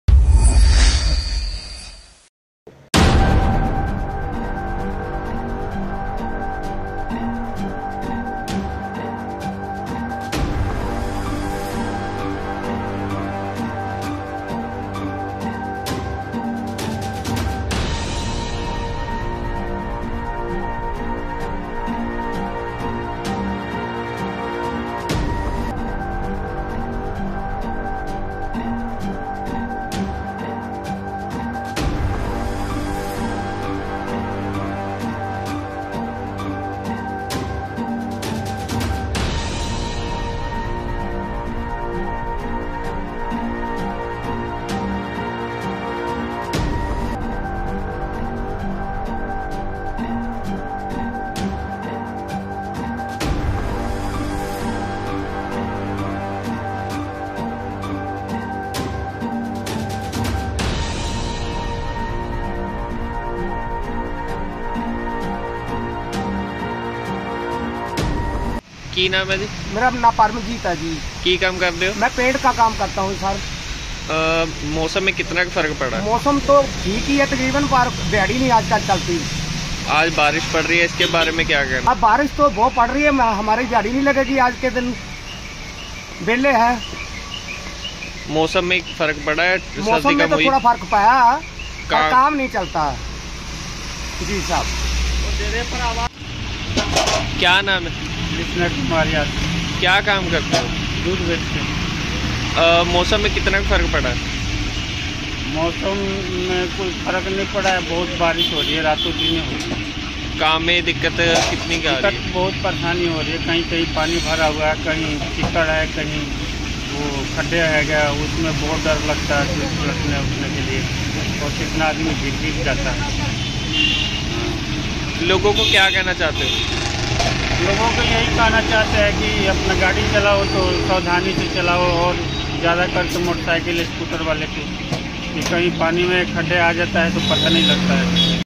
बारिश से रेलवे स्टेशन पर छाया अंधेरा, शहर में पानी खड़ा होने से लोग परेशान
Encounter News